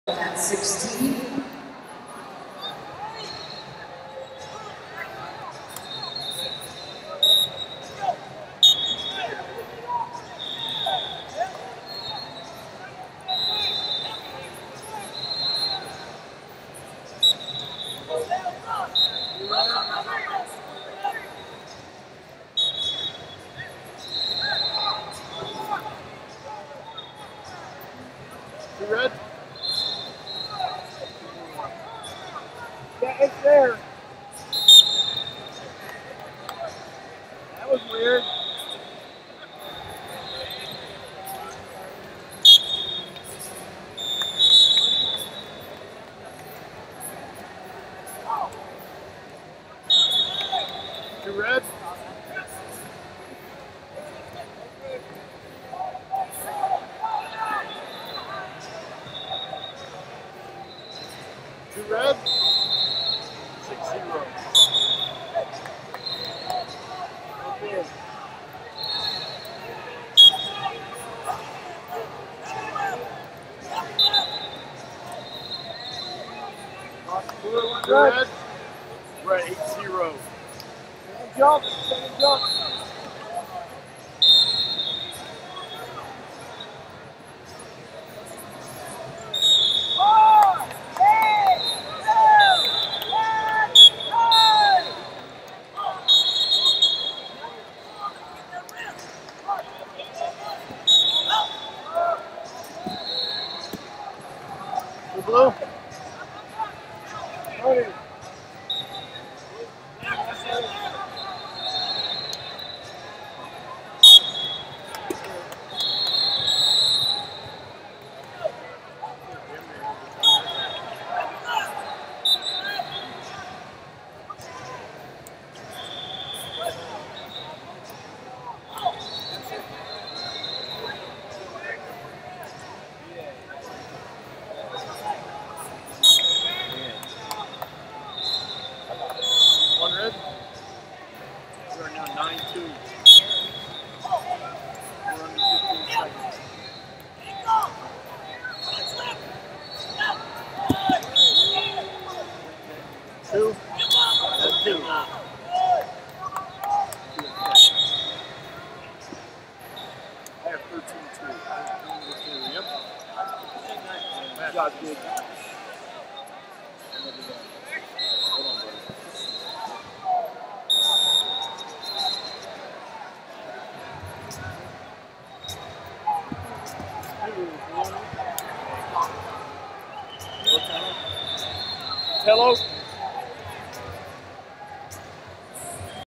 106 Lbs Round Of 32 - Jake Knight, Iowa Vs Jack Kazalas, Pennsylvania 49f6
FloSports/2021 US Marine Corps Junior And 16U National Championships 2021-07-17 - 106 Lbs Round Of 32 - Jake Knight, Iowa Vs Jack Kazalas, Pennsylvania 49f6